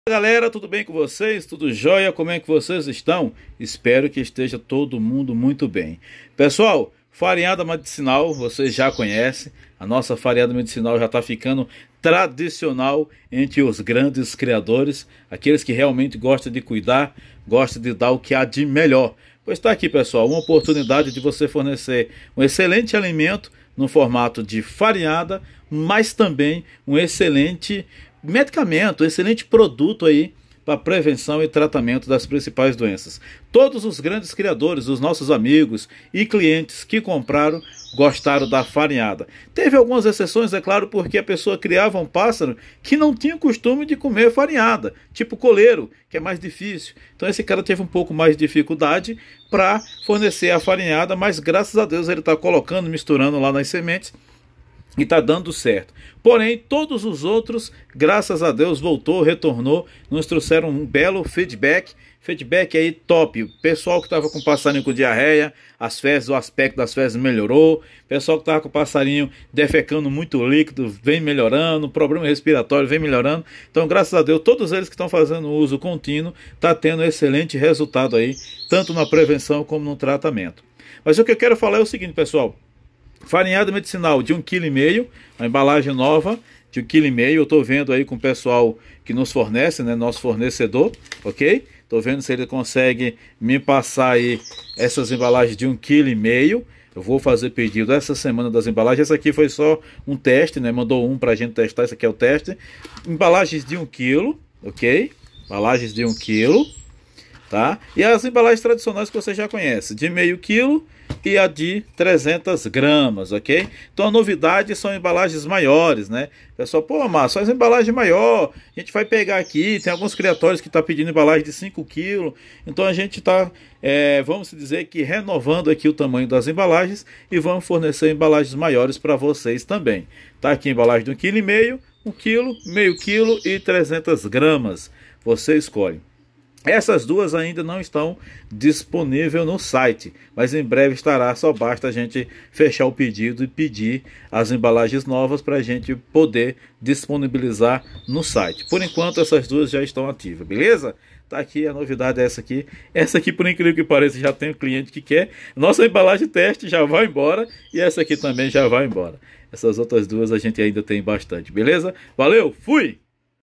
sucesso de venda, Aprovada por vários criadores na prevenção e tratamento de várias doenças em Aves
para adquirir a nossa farinhada medicinal para pássaros e aves em geral é muito fácil é só acessar o link abaixo.

aprovada por diversos criadores na prevenção e tratamento do peito seco, diarreia, problemas respiratórios, problemas na pele e infecções virais, bacteriana, fúngicas, e parasitárias.